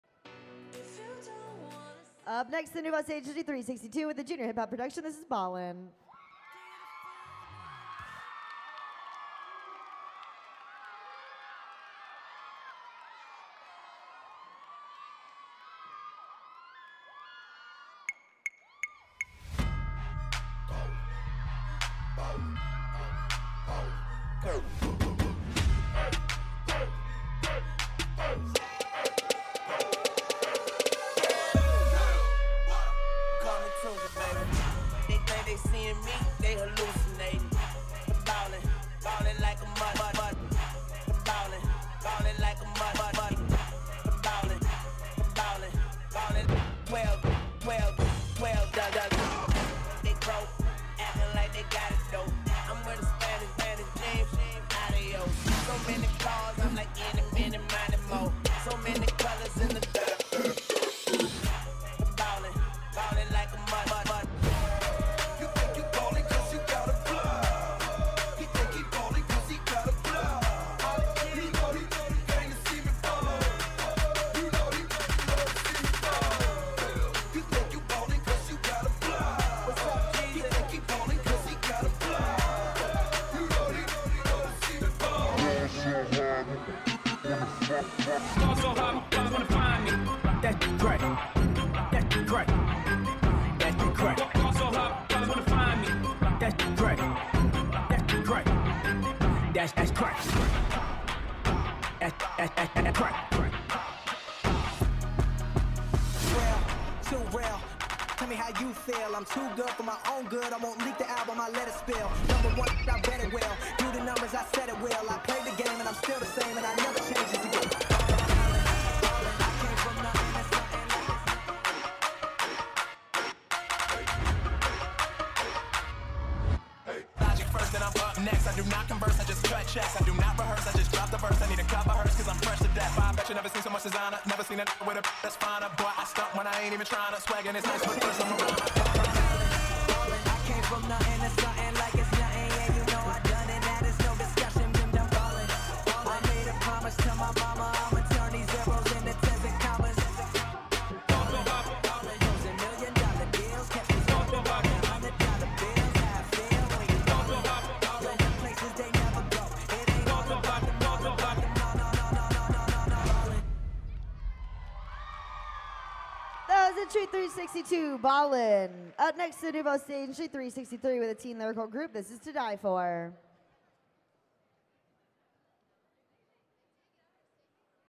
Ballin - Prodigy Dance and Performing Arts Centre
Nuvo Dallas, TX
Junior Hip-Hop Production
December 3, 2022
2nd Place Overall